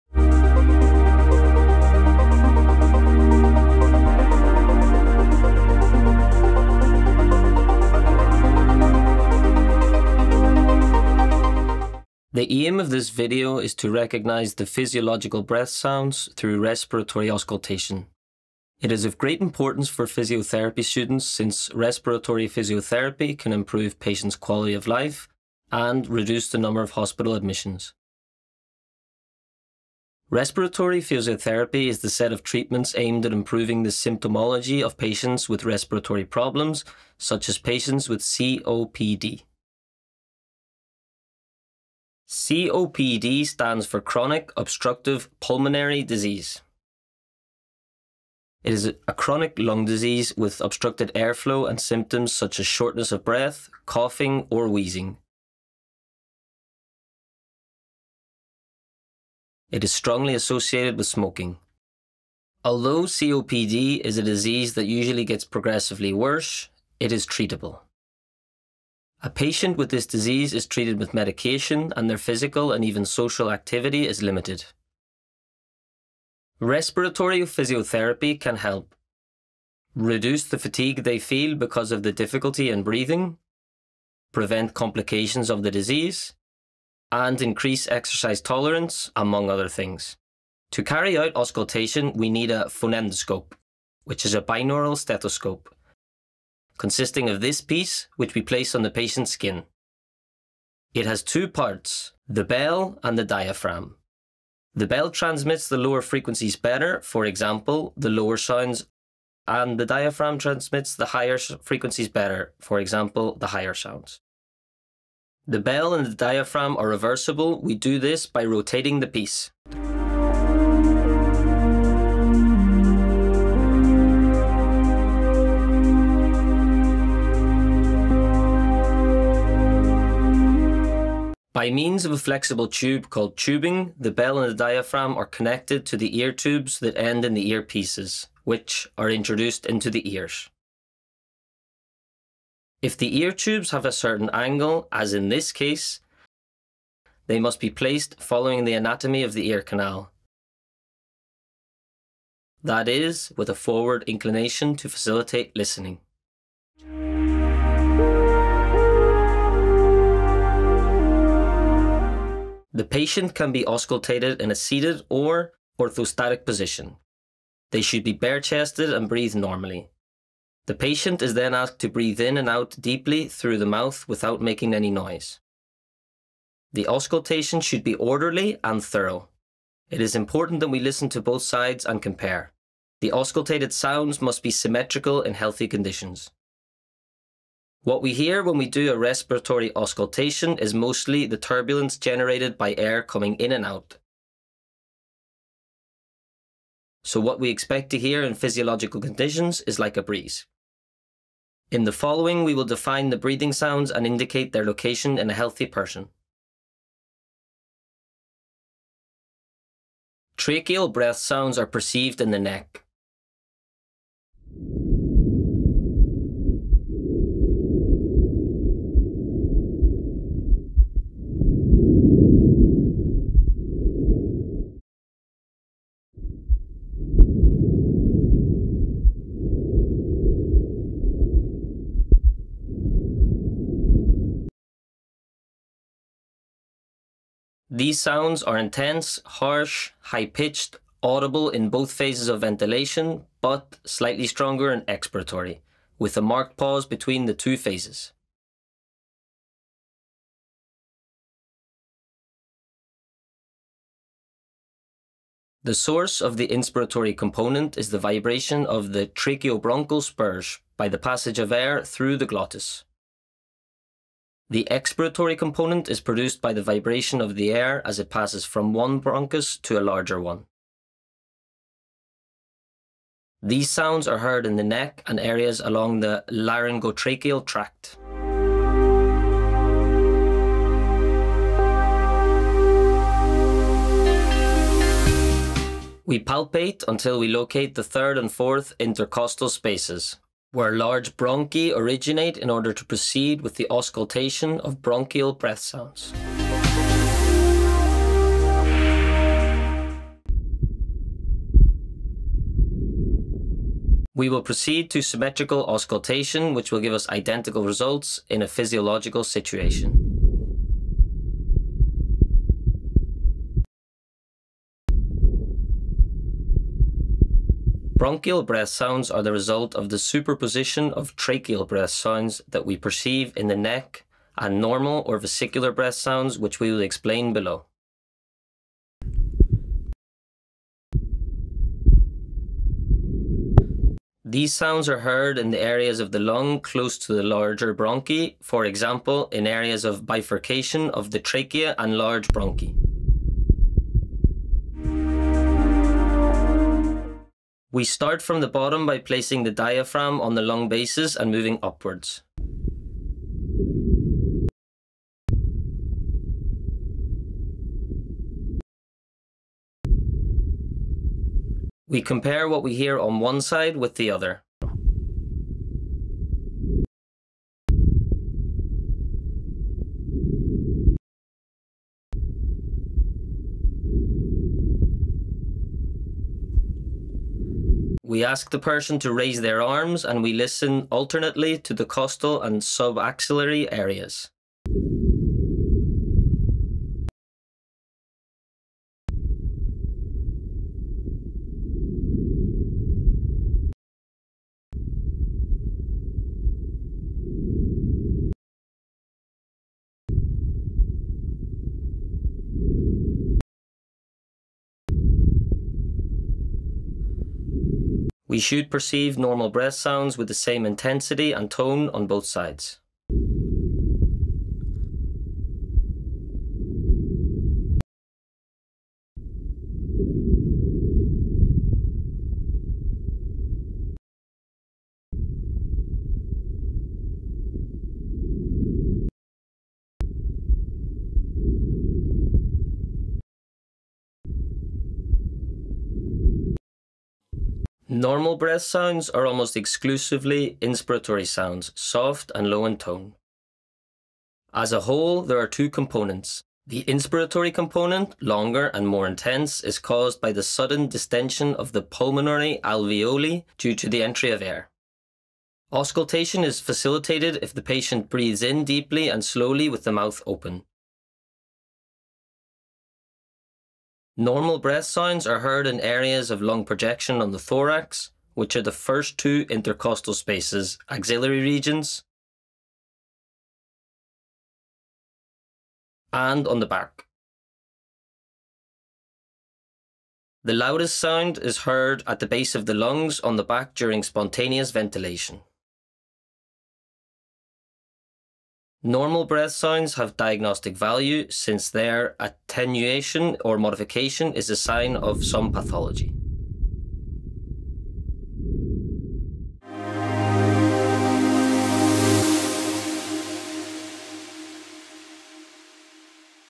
Respiratory auscultation: physiological breath sounds
Authors: María Dolores Mauricio /Eva Serna, Vannina González, Solanye Guerra, Antonio Alberola, María Àngels Cebrià, Francisco Martínez, Andrea Fortea, Jude Moreland

Abstract: This video shows how to recognize the physiological breath sounds through respiratory auscultation. Suitable for students of health sciences.

                    Taller d'Audiovisuals de la Universitat de València